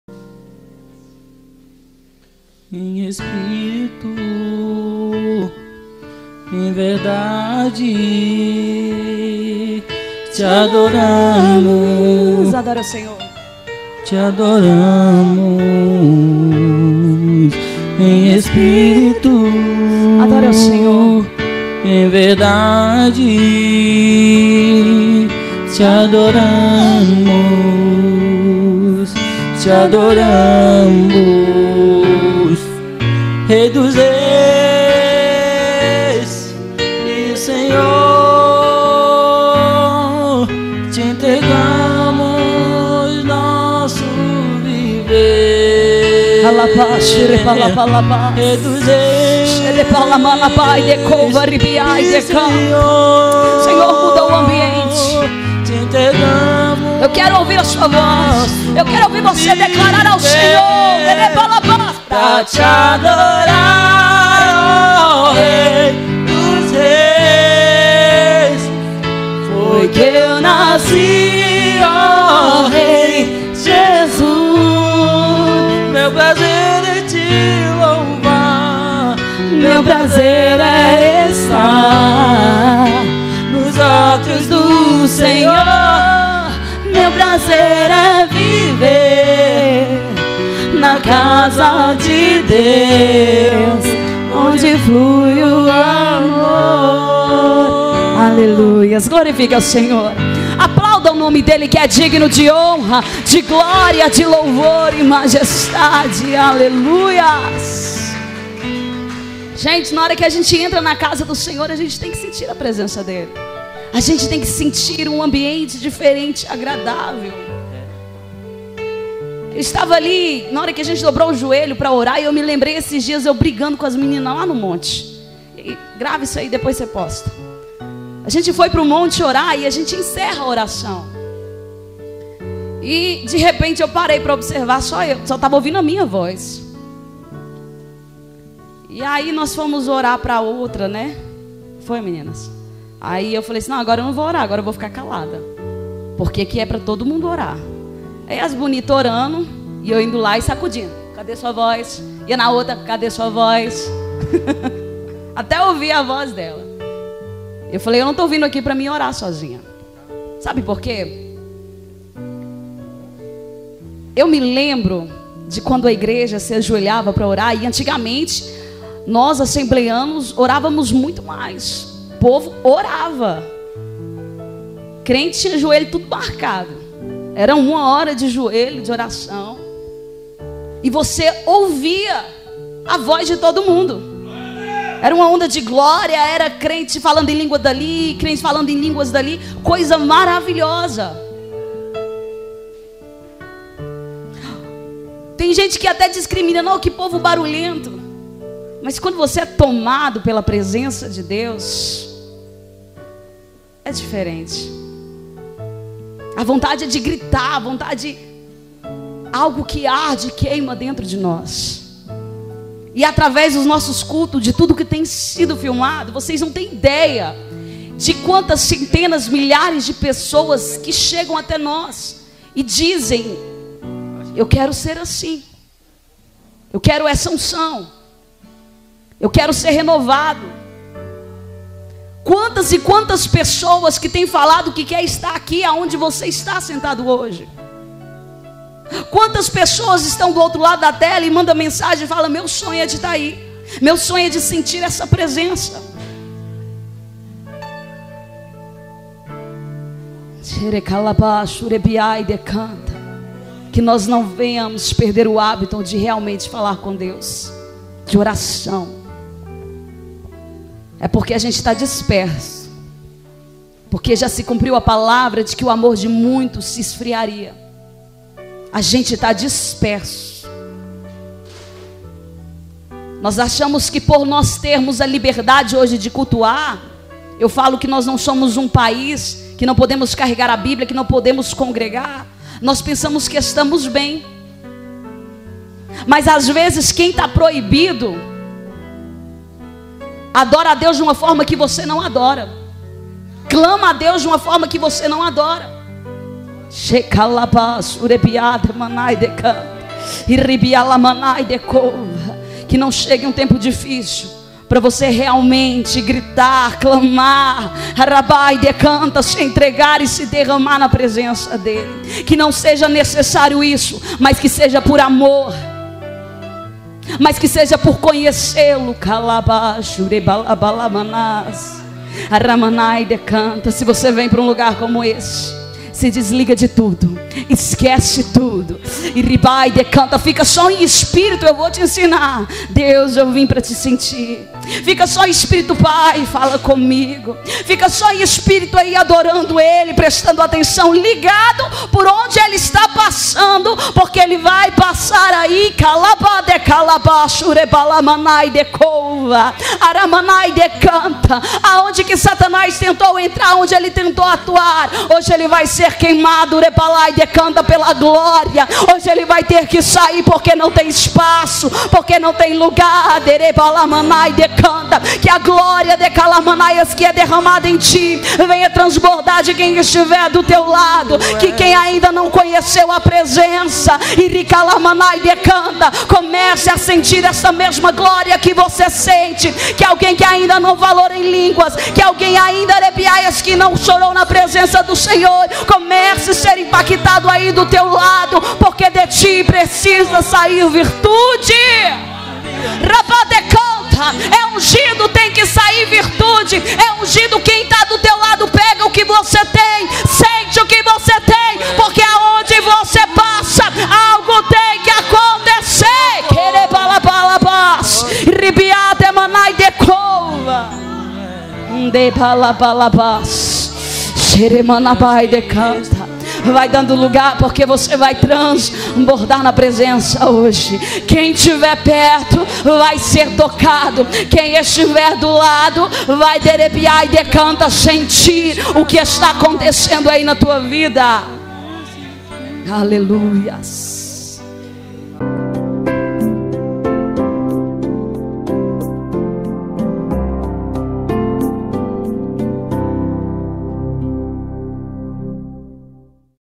Ester Ribeiro - LOUVOR - Culto na Roça 2020
Uma vigília poderosa Culto na Roça   realizada na cidade de Caldazinha no estado de Goias
Idealizadora Prª Ester Ribeiro